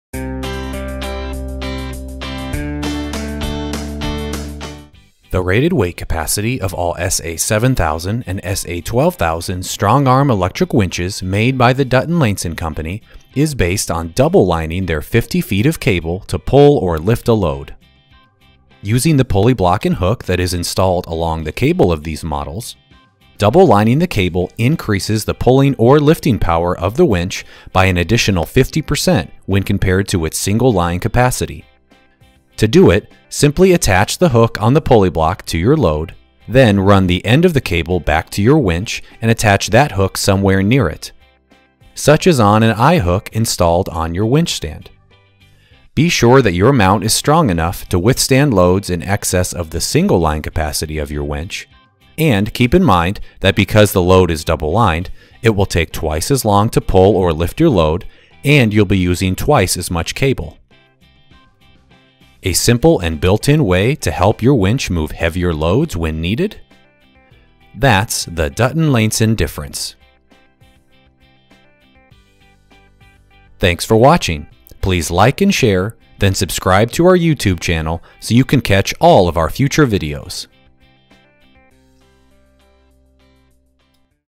Double-Lining Cable on Electric Winches - DL Difference
People often are confused about the idea of double-lining cable on a StrongArm® electric winch.  Seeing it being done in this video should provide some clarity.

0:00 Outro
0:05 Double-lining cable on electric winches
1:13 Outro

Here is the transcript of this video:

"The rated weight capacity of all SA7000 and SA12000 StrongArm electric winches made by the Dutton-Lainson Company is based on double-lining their 50 feet of cable to pull or lift a load.

Using the pulley block and hook that is installed along the cable of these models, double-lining the cable increases the pulling or lifting power of the winch by an additional 50% when compared to its single line capacity.

To do it, simply attach the hook on the pulley block to your load, then run the end of the cable back to your winch and attach that hook somewhere near it, such as on an eyehook installed on your winch stand.

Be sure that your mount is strong enough to withstand loads in excess of the single-line capacity of your winch, and keep in mind that because the load is double-lined, it will take twice as long to pull or lift your load and you'll be using twice as much cable.

A simple and built-in way to help your winch move heavier loads when needed?  That's the Dutton-Lainson difference!